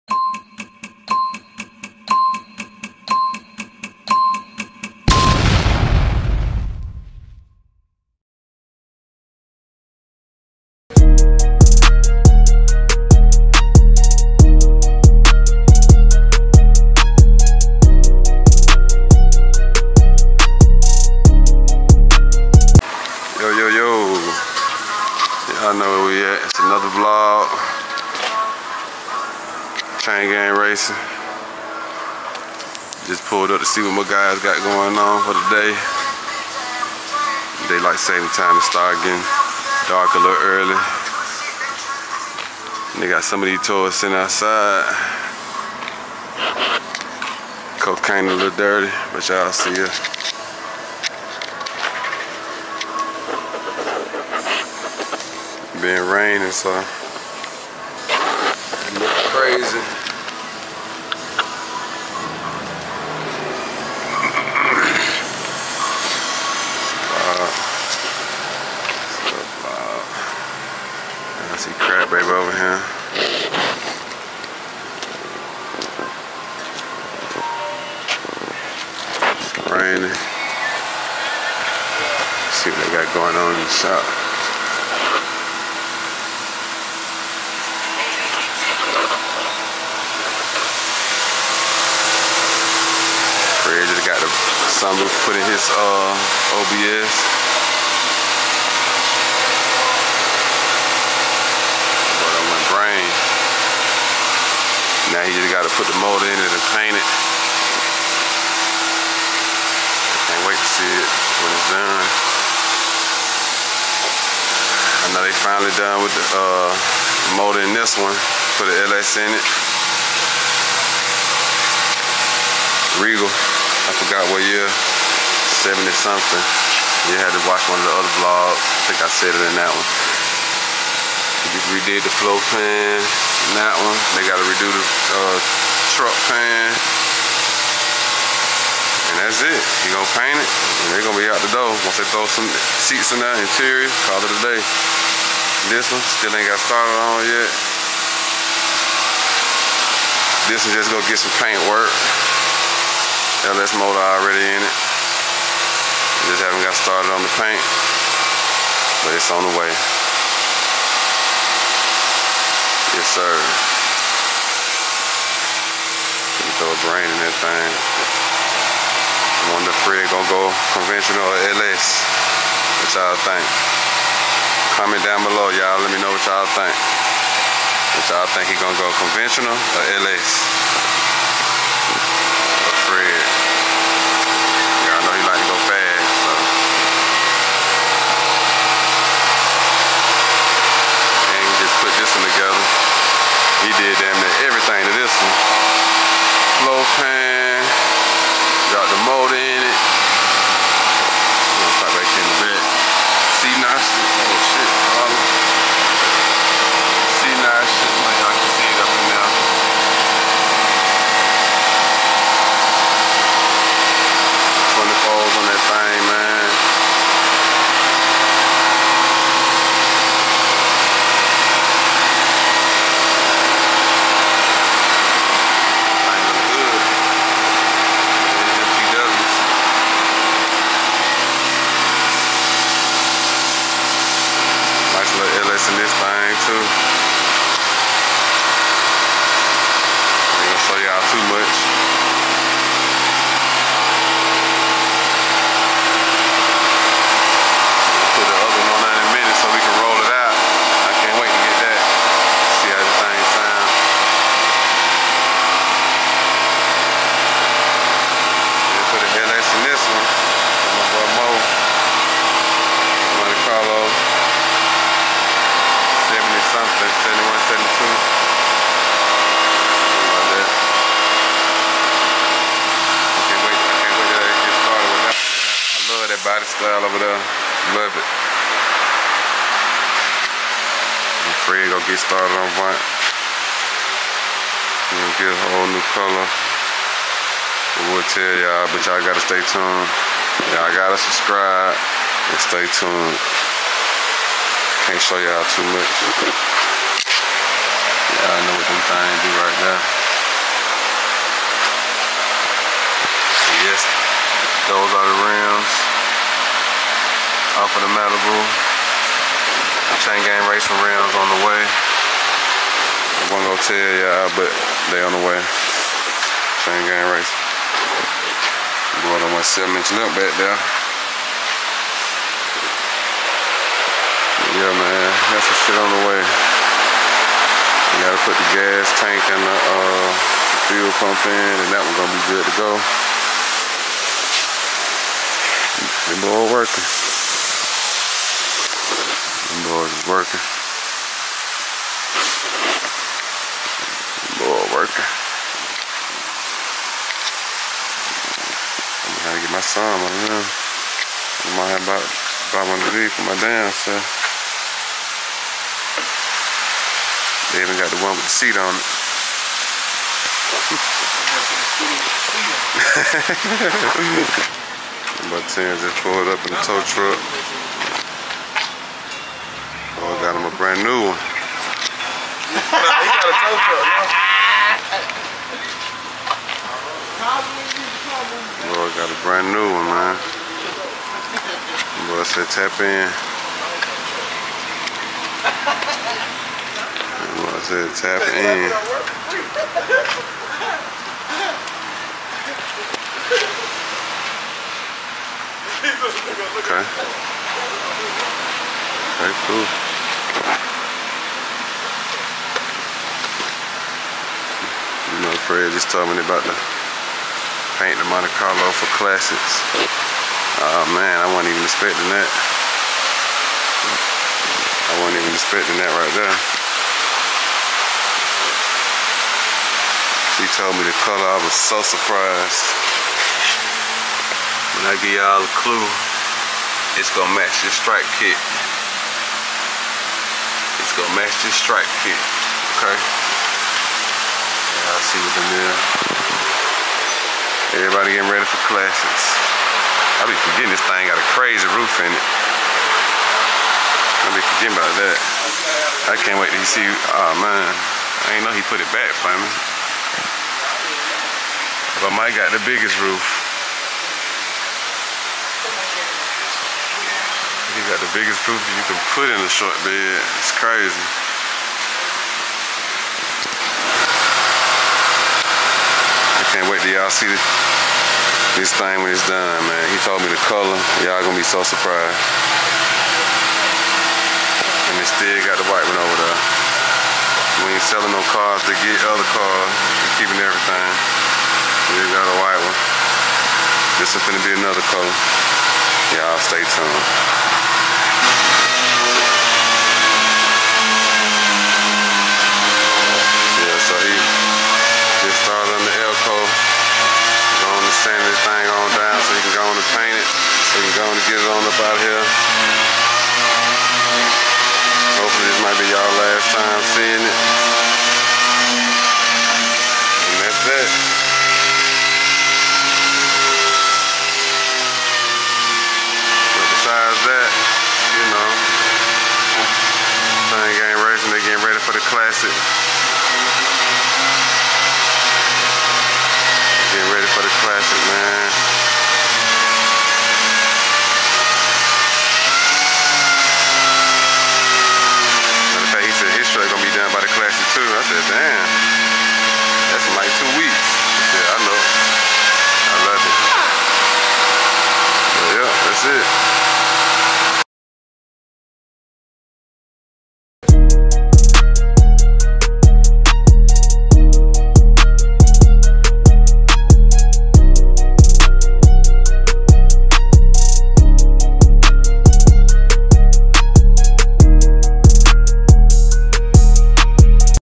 Chain Gang Racing Shop Vlog Episode 7!
Another shop vlog of the guys working at the shop getting the whips ready for the classic! @CHAINGANGRACING